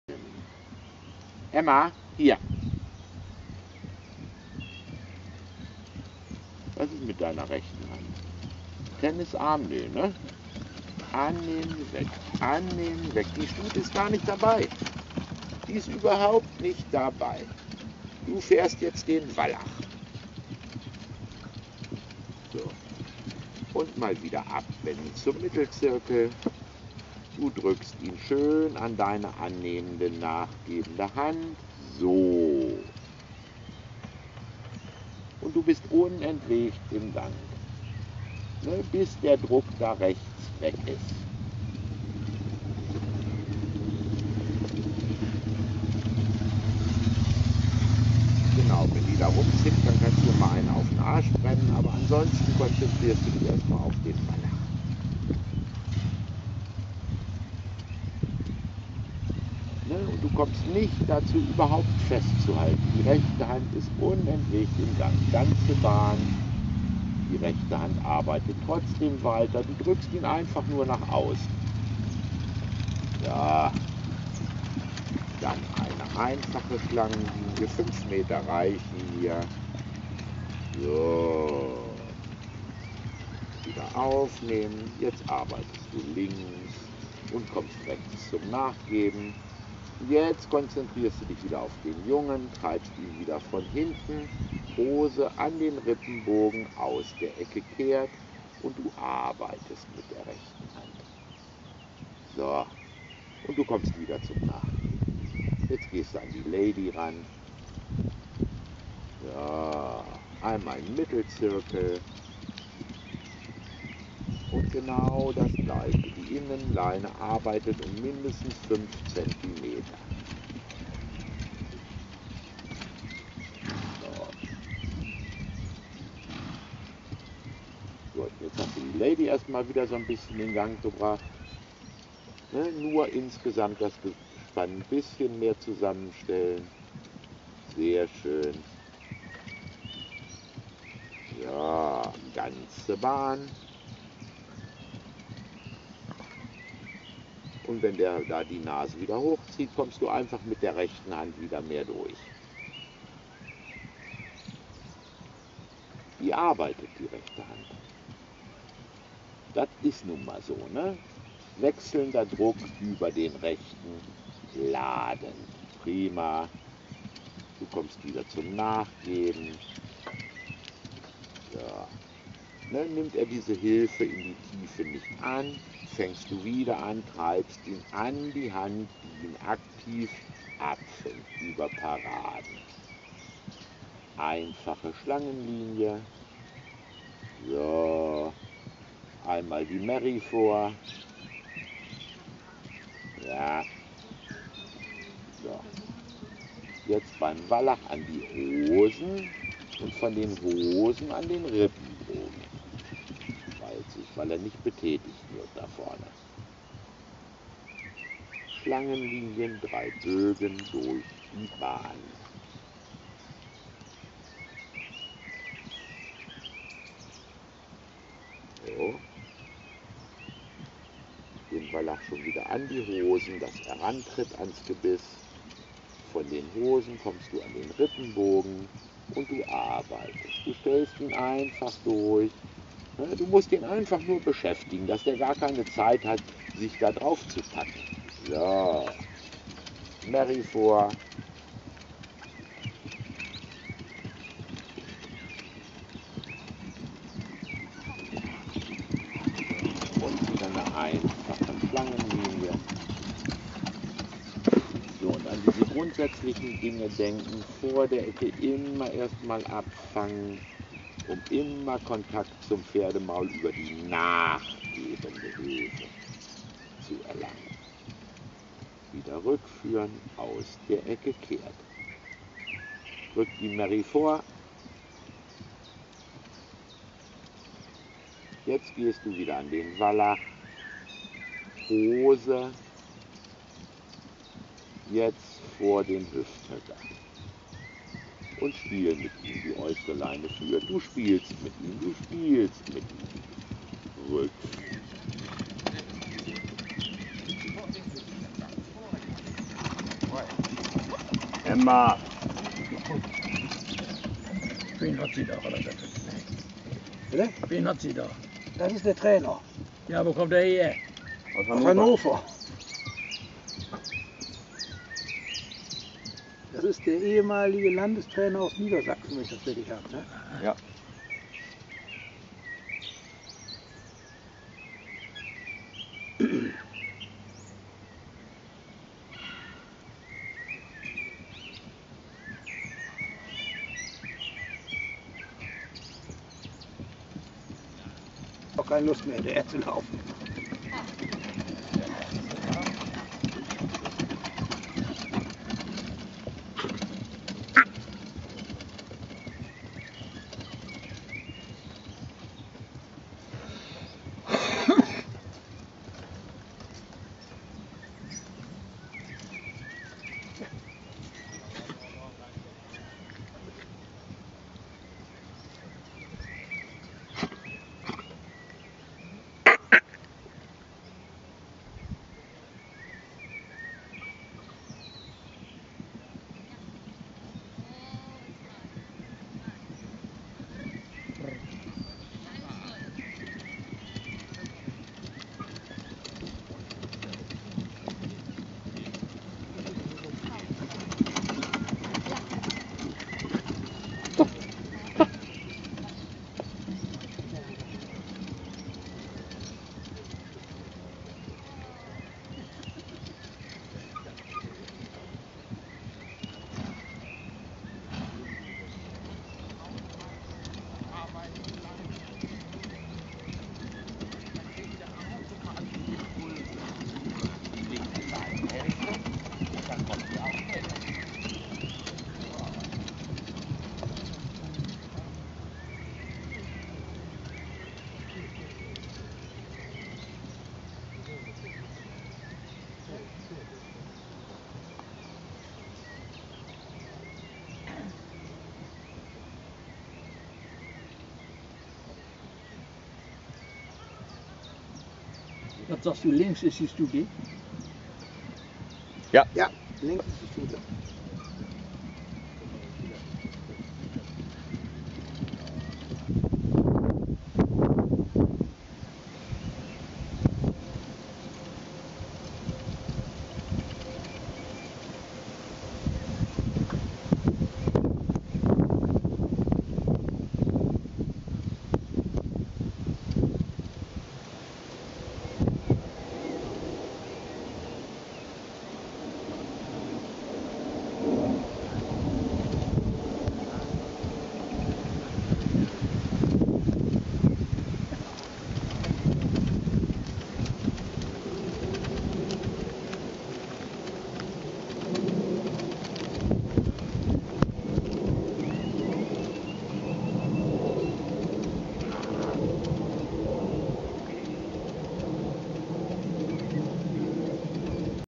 2011-06-11 Fahrtraining auf dem Fohlenhof Anja mit Furie und Florentine-Desktop.m4v
Fahrtraining auf dem Fohlenhof mit Ullrich Altekruse - Trainer A, Richter und Parcourschef